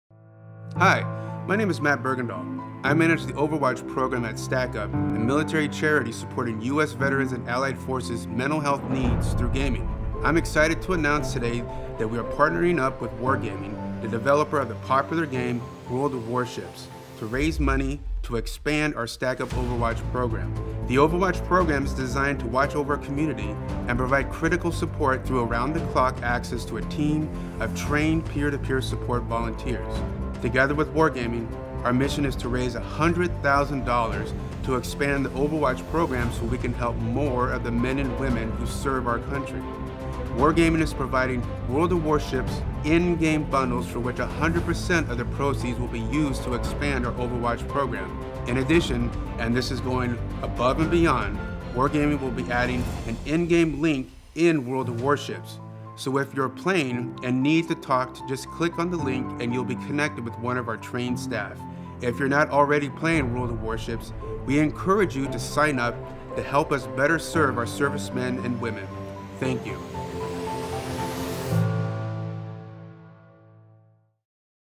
Wargaming - Stack Up - Operation Lifeboat
Wargaming, the developer of World of Warships, and Stack Up, a nonprofit supporting veterans through gaming, today announced a partnership to raise money and awareness for military veterans in need of mental health support.